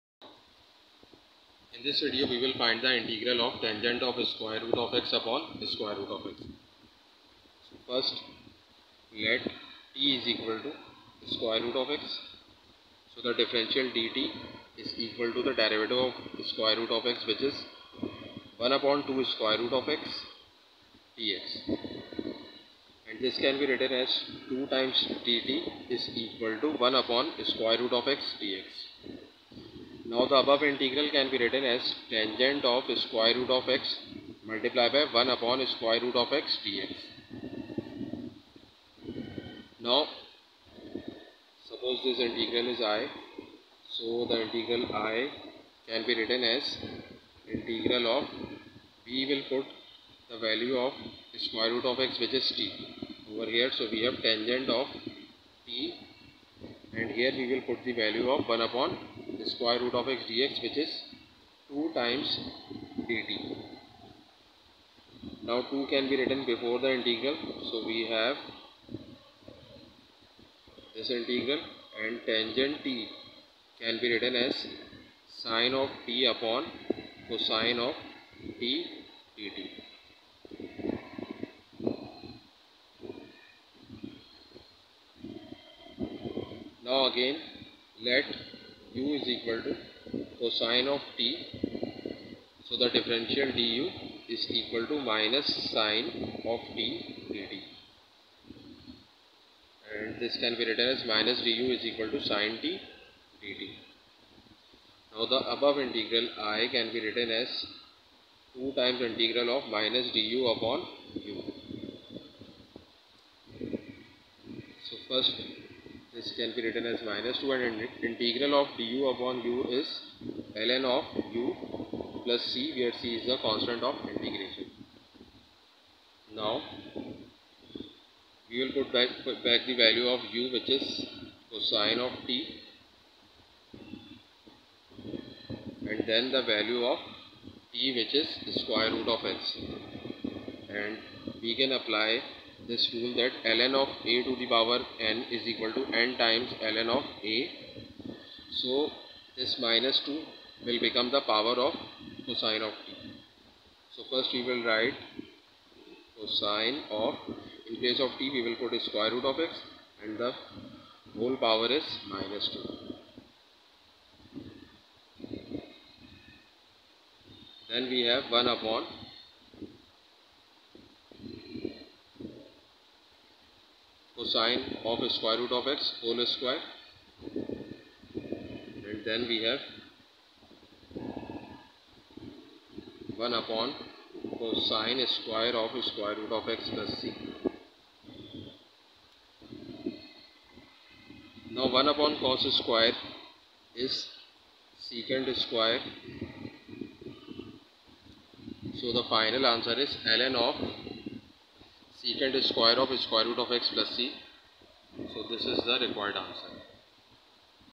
Integral of (tan√x)/√x | Integration of Trigonometric Functions
In this video we will find the integral of trigonometric function